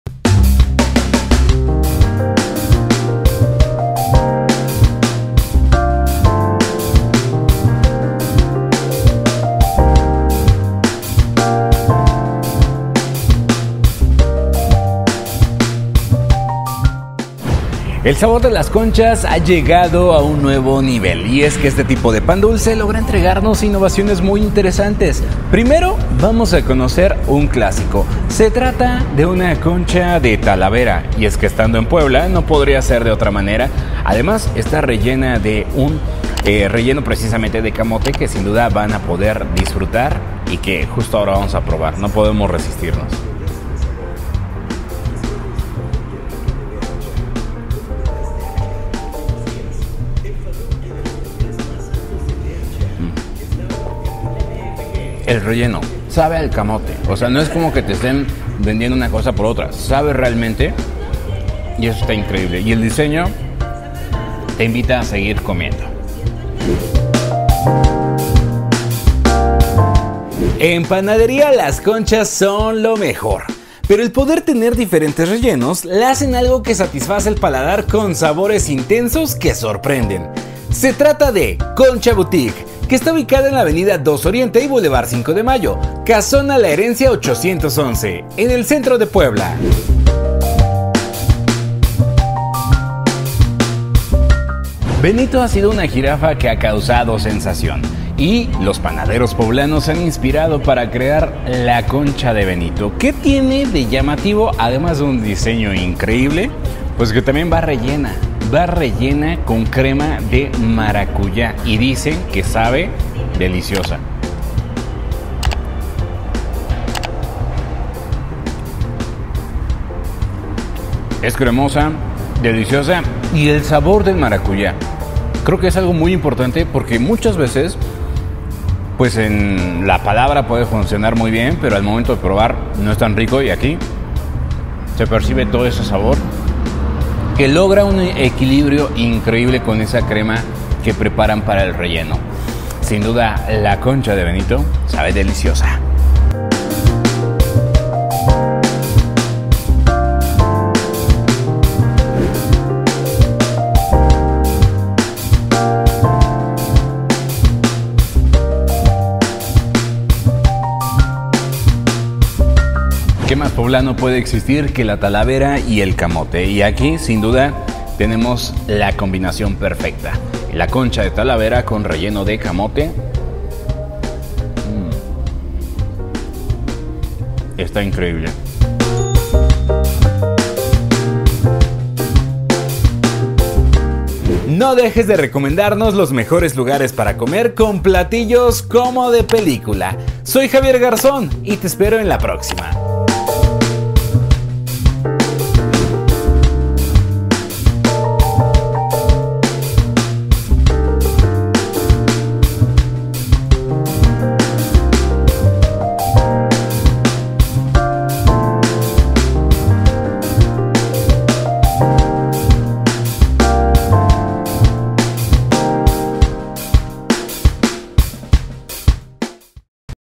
Concha Boutique - Javier Garzón
Probé la concha inspirada en la Jirafa Benito, que hace poco fue rescatada y llegó a Africam Safari, la concha esta rellena de maracuyá y su sabor esta increíble, acompáñame a disfrutar de esta deliciosa creación en Puebla